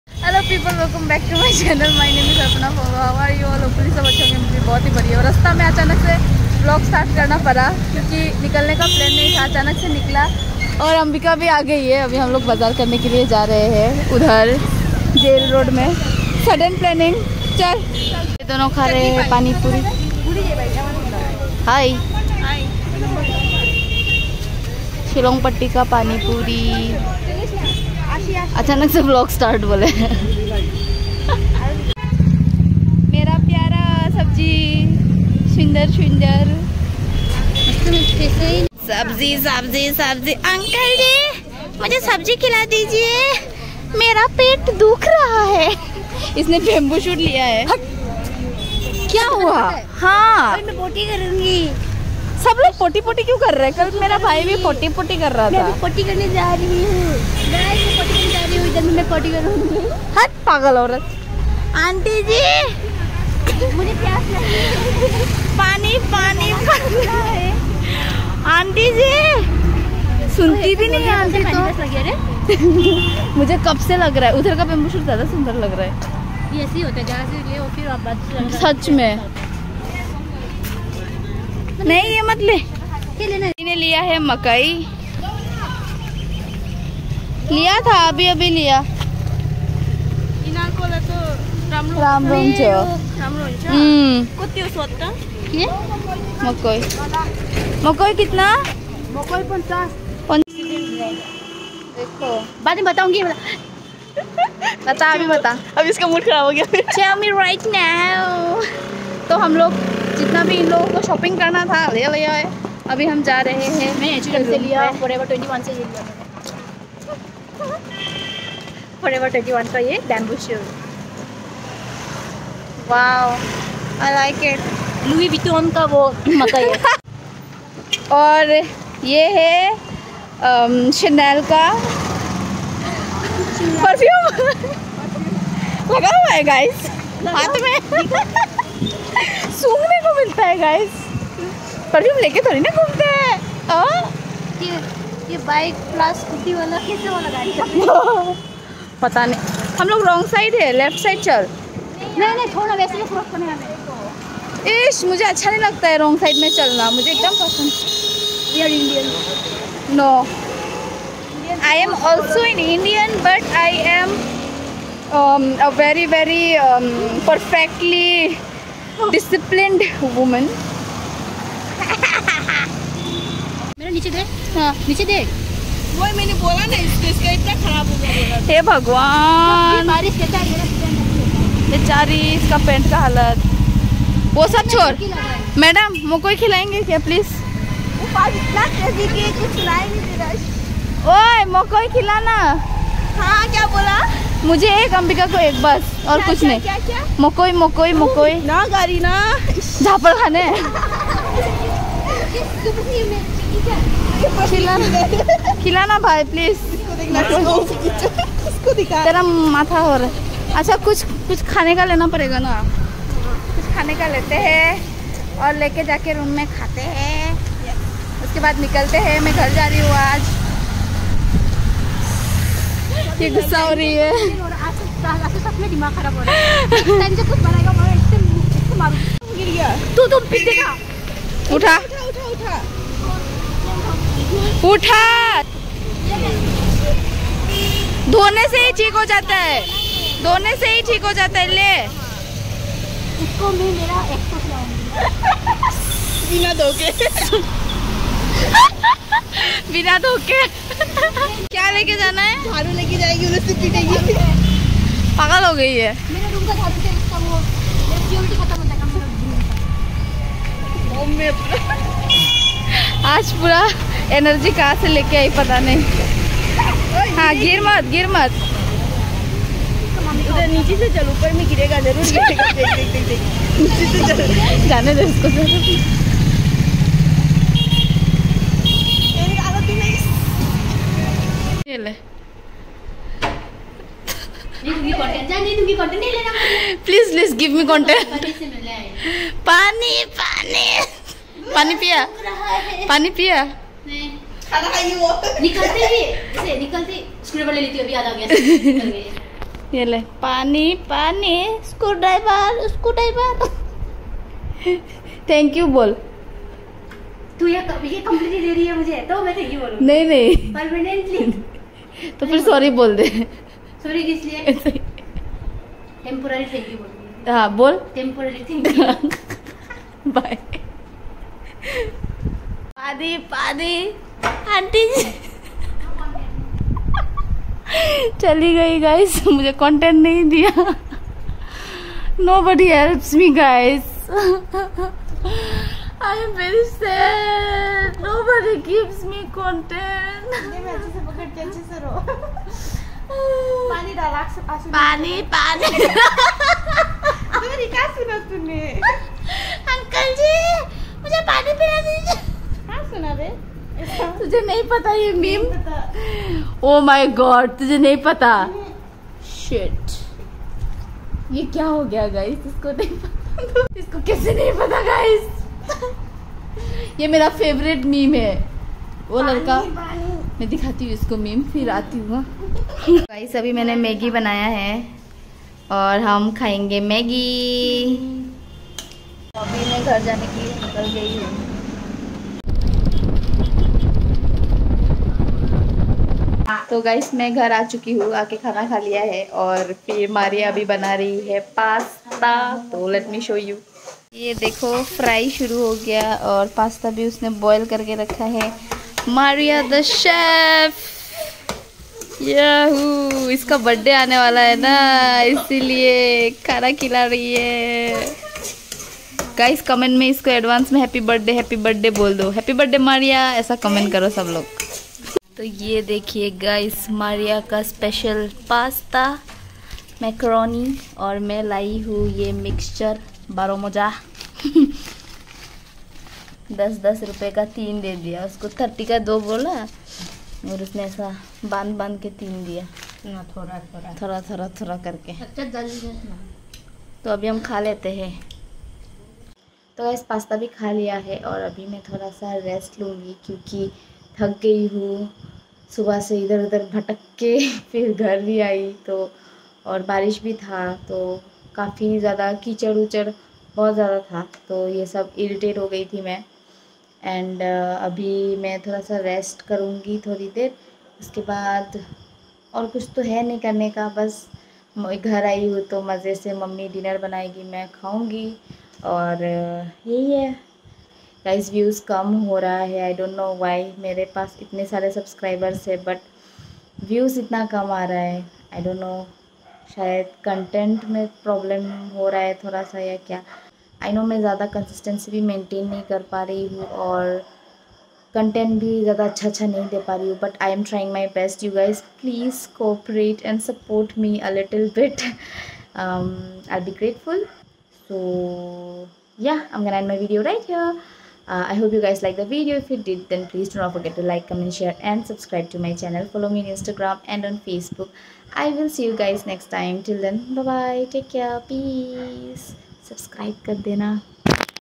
rasta se uthaya bambooshoot kisko khilayegi ambika | mujhe rula diya😭 | Day 190 | Arpana Phonglo
If you like the video then do like this video and